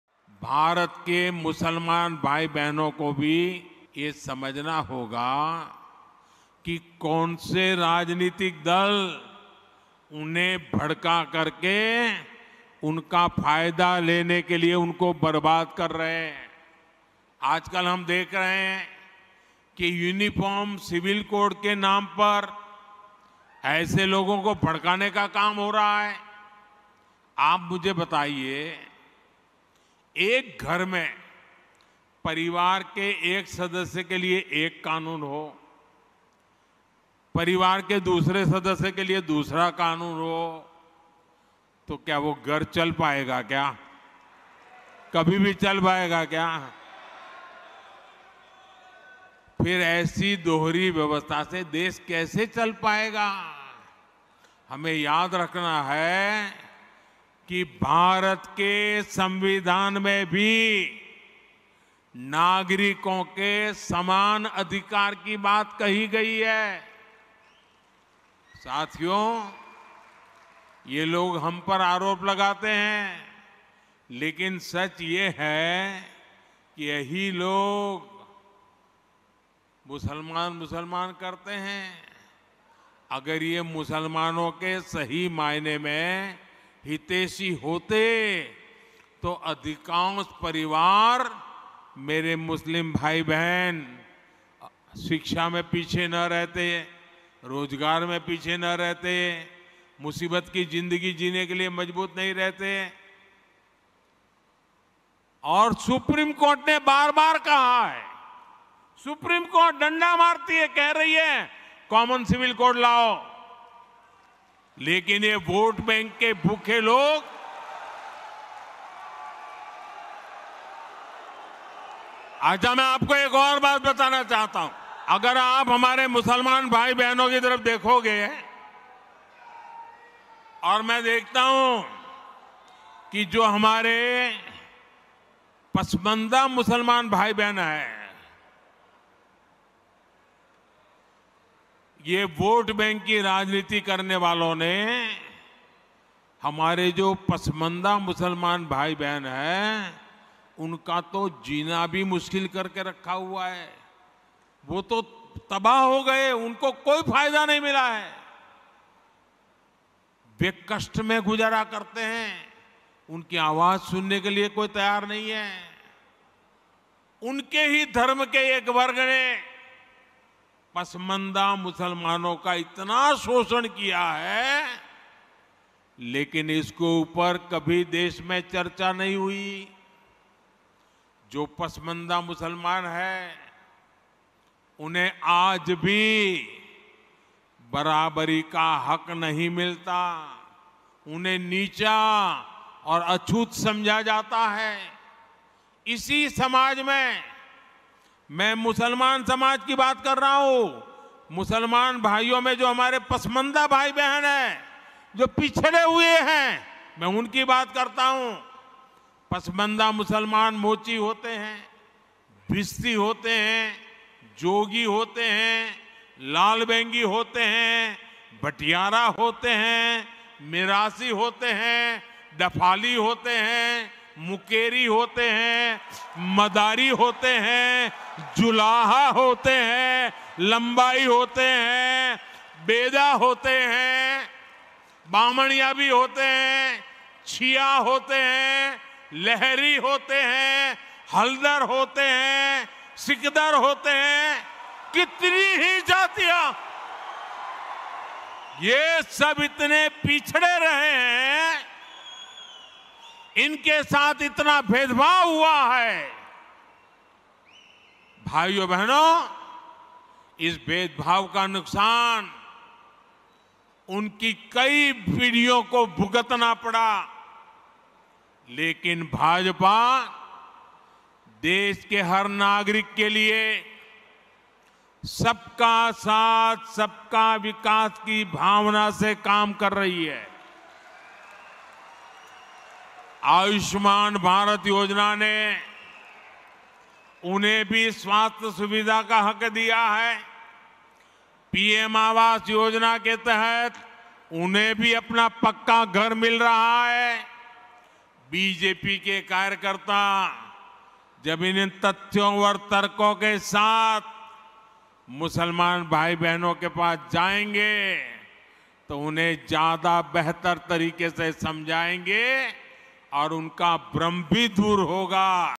PM Modi ON UCC: कॉमन सिविल कोड पर मोदी का ऐलान- एक देश, एक कानून, 3 तलाक का इस्लाम से नहीं संबंध
प्रधानमंत्री नरेंद्र मोदी आज भोपाल दौरे पर हैं. जहां उन्होंने 5 वंदे भारत एक्सप्रेस को हरी झंडी दिखाई. वहीं पीएम ने तीन तलाक पर बयान देते हुए कहा कि मुस्लिम समाज में गलत भ्रम फैलाया जा रहा है. प्रधानमंत्री ने कहा कि कॉमन सिविल को लेकर बीजपी भ्रम दूर करेगी. (PM Modi ON UCC)